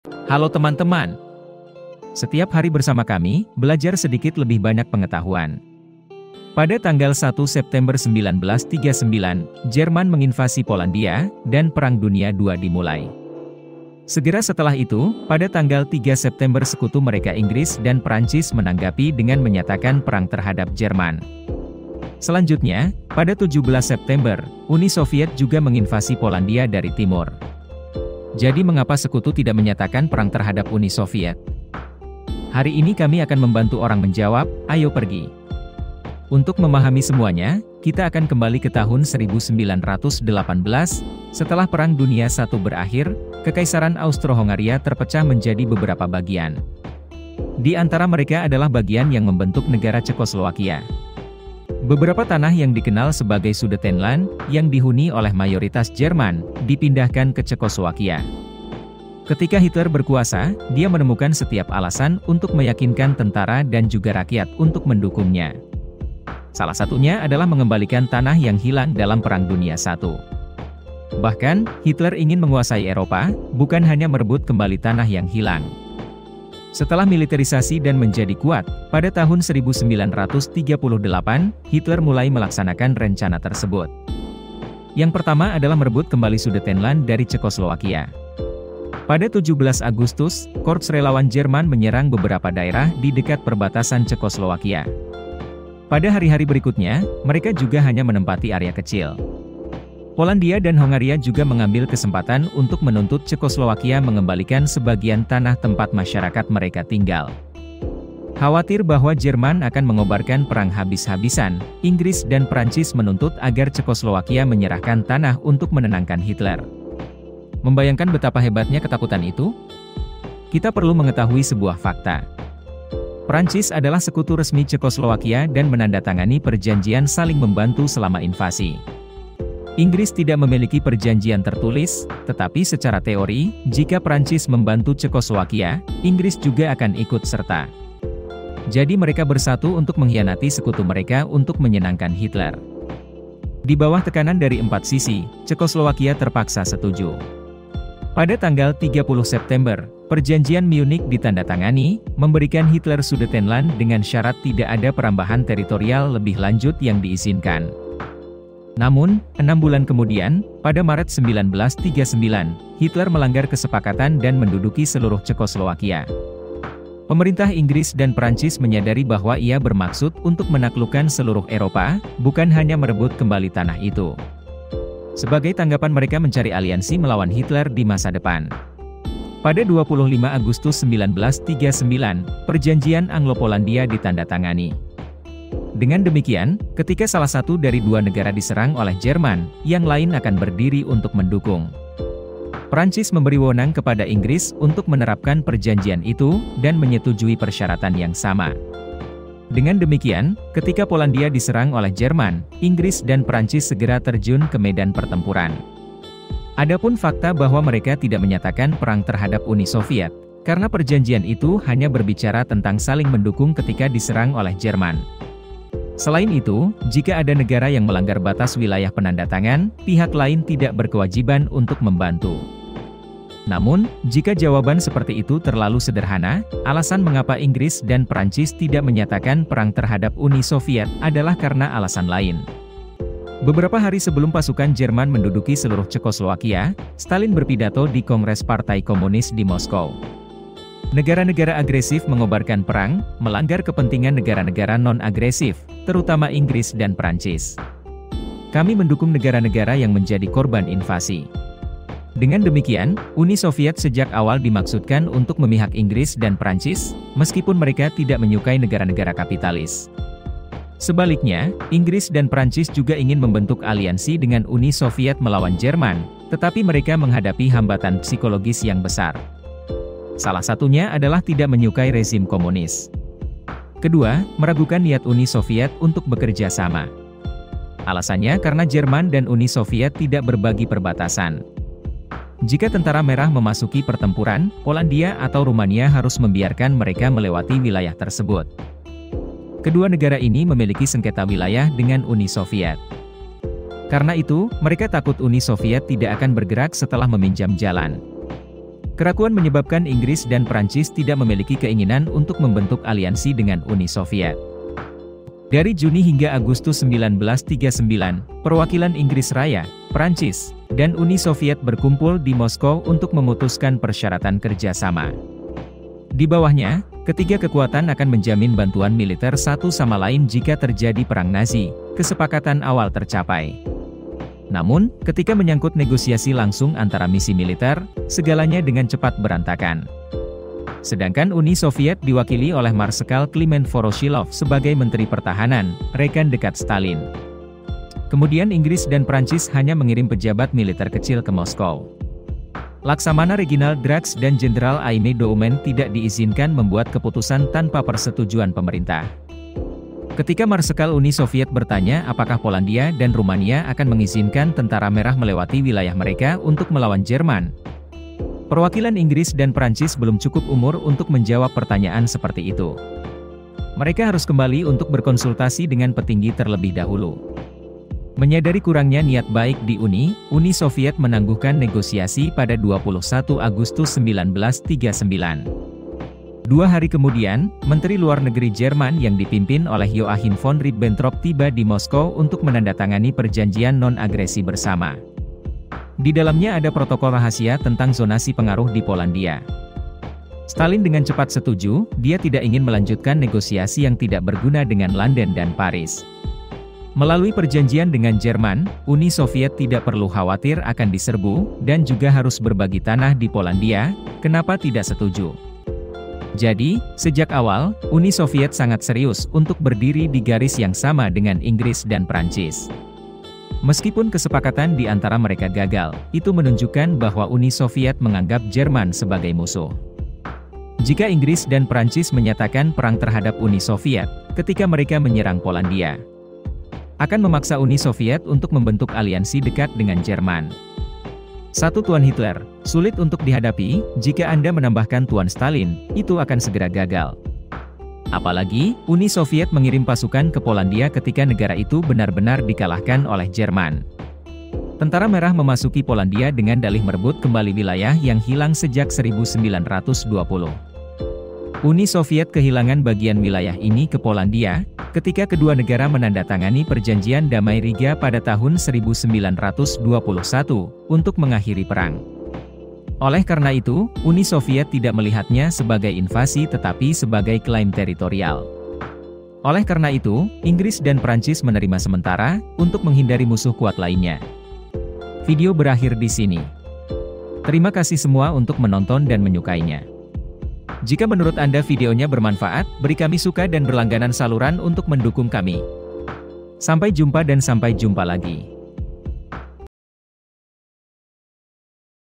Mengapa Sekutu tidak menyatakan perang terhadap Uni Soviet, ketika Polandia diserang oleh Uni Soviet
Halo teman teman! Setiap hari bersama kami, belajar sedikit lebih banyak pengetahuan. Pada tanggal 1 September 1939, Jerman menginvasi Polandia, dan Perang Dunia 2 dimulai. Segera setelah itu, pada tanggal 3 September sekutu mereka Inggris dan Prancis menanggapi dengan menyatakan perang terhadap Jerman. Selanjutnya, pada 17 September, Uni Soviet juga menginvasi Polandia dari timur. Jadi mengapa Sekutu tidak menyatakan perang terhadap Uni Soviet? Hari ini kami akan membantu orang menjawab, Ayo pergi.
Untuk memahami semuanya, Kita akan kembali ke tahun 1918, setelah Perang Dunia 1 berakhir, Kekaisaran Austro-Hongaria terpecah menjadi beberapa bagian. Di antara mereka adalah bagian yang membentuk negara Cekoslowakia. Beberapa tanah yang dikenal sebagai Sudetenland, yang dihuni oleh mayoritas Jerman, dipindahkan ke Cekoslowakia.
Ketika Hitler berkuasa, dia menemukan setiap alasan untuk meyakinkan tentara dan juga rakyat untuk mendukungnya. Salah satunya adalah mengembalikan tanah yang hilang dalam Perang Dunia 1. Bahkan, Hitler ingin menguasai Eropa, bukan hanya merebut kembali tanah yang hilang.
Setelah militerisasi dan menjadi kuat, pada tahun 1938, Hitler mulai melaksanakan rencana tersebut. Yang pertama adalah merebut kembali Sudetenland dari Cekoslowakia. Pada 17 Agustus, Korps Relawan Jerman menyerang beberapa daerah di dekat perbatasan Cekoslowakia.
Pada hari-hari berikutnya, mereka juga hanya menempati area kecil. Polandia dan Hongaria juga mengambil kesempatan untuk menuntut Cekoslowakia mengembalikan sebagian tanah tempat masyarakat mereka tinggal.
Khawatir bahwa Jerman akan mengobarkan perang habis-habisan, Inggris dan Prancis menuntut agar Cekoslowakia menyerahkan tanah untuk menenangkan Hitler. Membayangkan betapa hebatnya ketakutan itu? Kita perlu mengetahui sebuah fakta.
Donasi untuk FYI:
(Semua donasi Anda akan membantu kami bertahan dan berkembang lebih jauh. Terima kasih banyak)
Sukai Video + Bagikan video dan daftar.